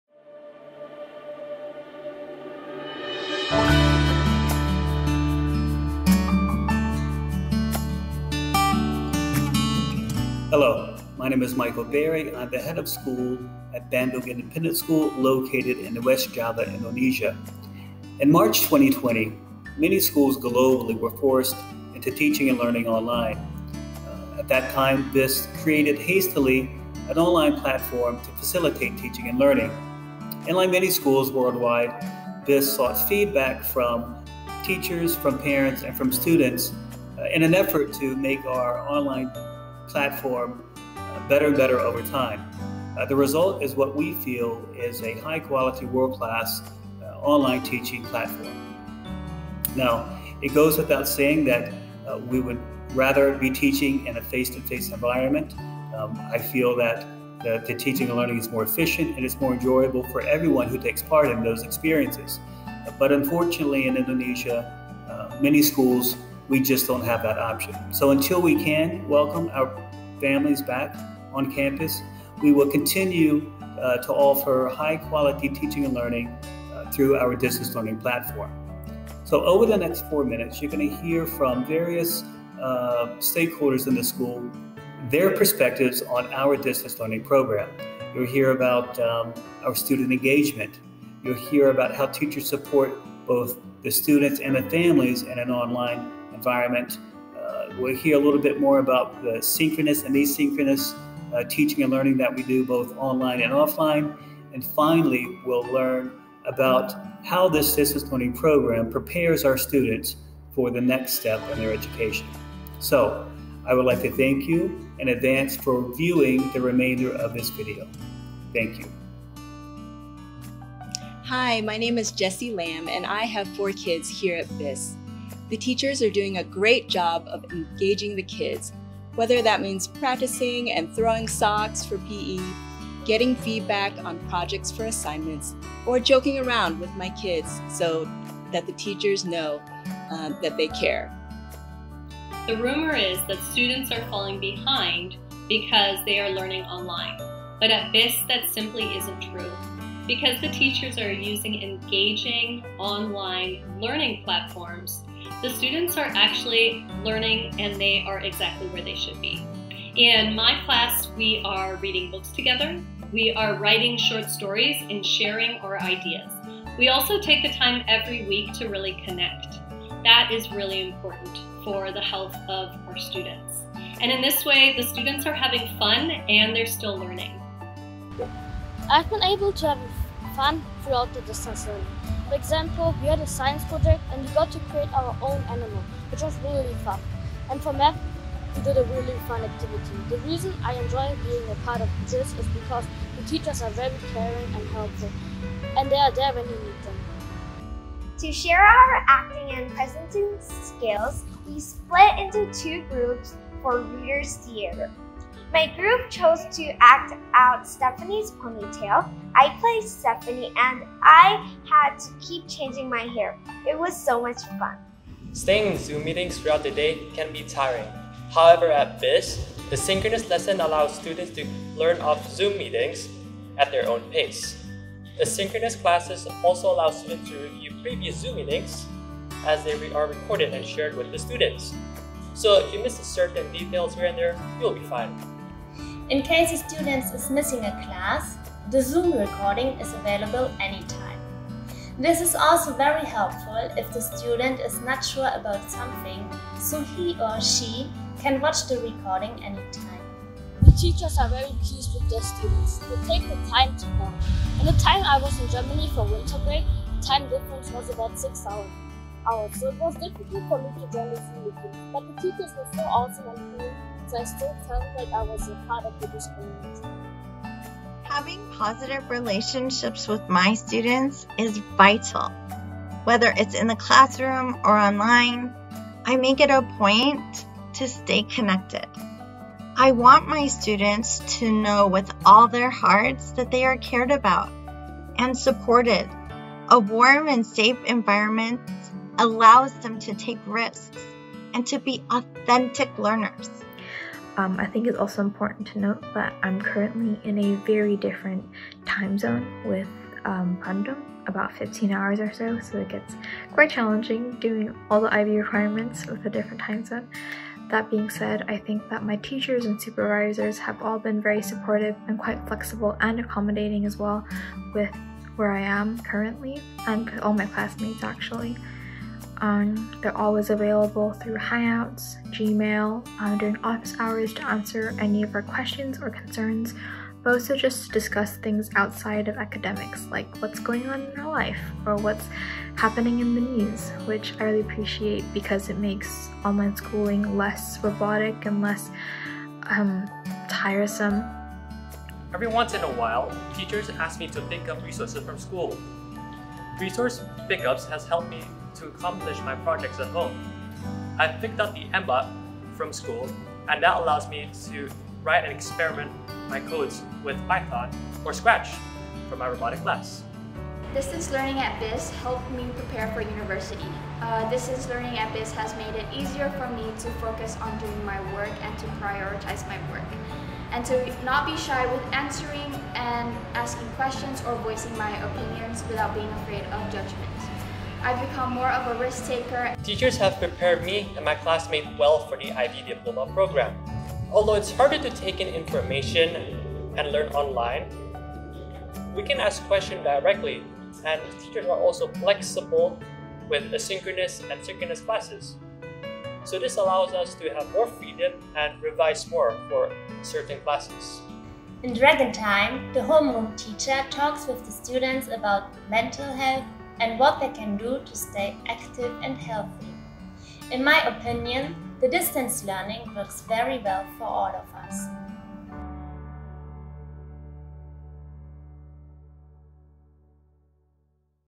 Online Learning Testimonial Bandung Independent School
One year ago on March 13, 2020, BIS began teaching and learning online.  Although we all feel that face-to-face learning is a more ideal approach to learning, schools in Indonesia do not have the option to conduct classes offline.

Over time, BIS has created a fabulous online learning platform by gaining feedback from various school stakeholders including parents, teachers, and students.  In the most recent survey, almost 90% of the parent community noted high student engagement and said the online platform supports students and families offline and online. Additionally, 85% of the parents rated our online learning platform “Good” or “Excellent”.

In this video, you can hear for yourself why the parents, students, and teachers feel that the BIS online platform is a wonderful tool to facilitate high-quality teaching and learning.